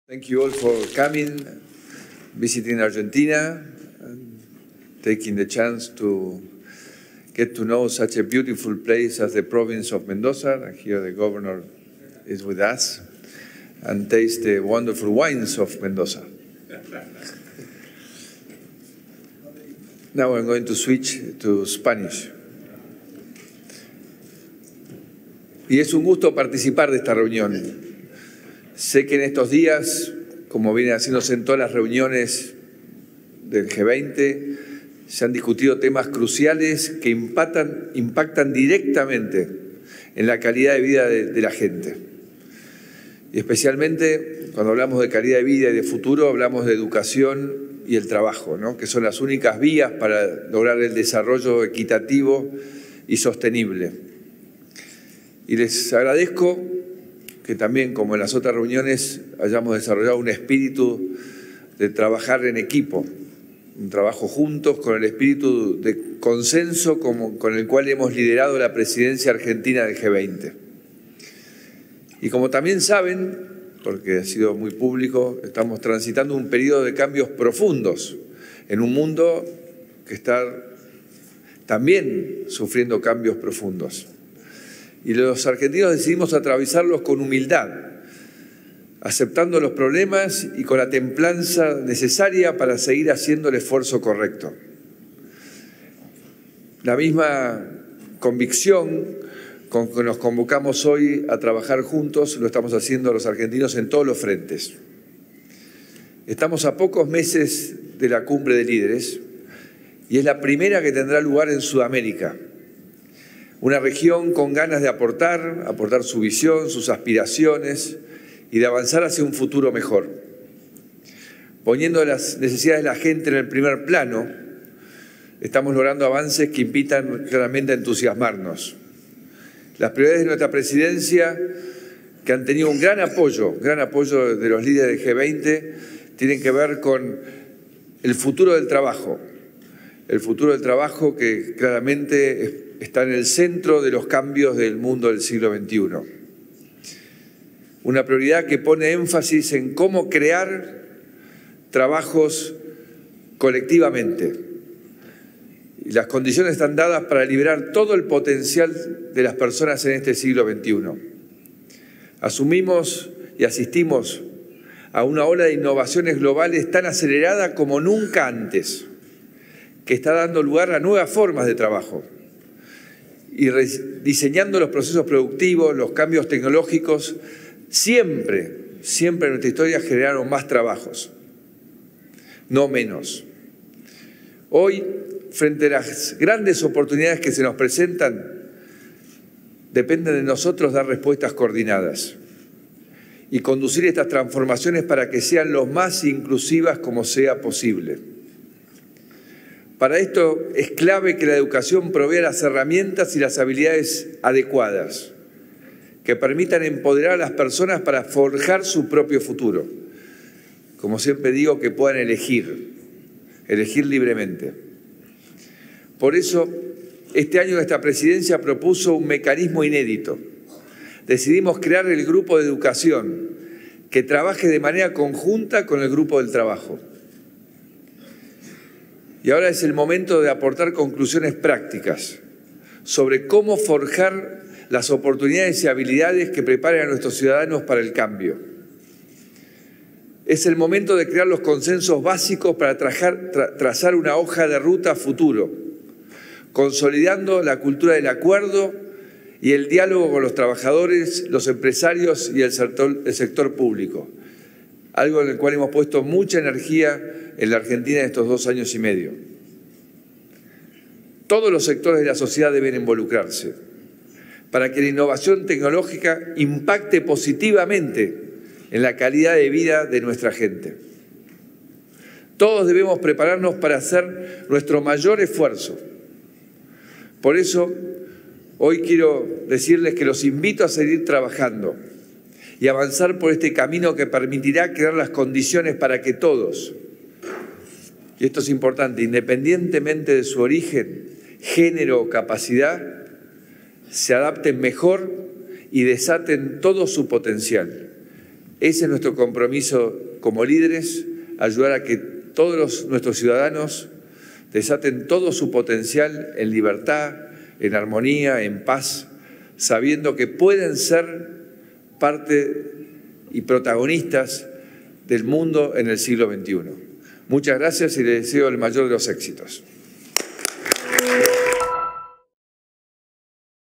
Macri en la cumbre del G20: "Transitamos un período de cambios profundos"
El presidente afirmó que éste "es el momento de crear consensos básicos"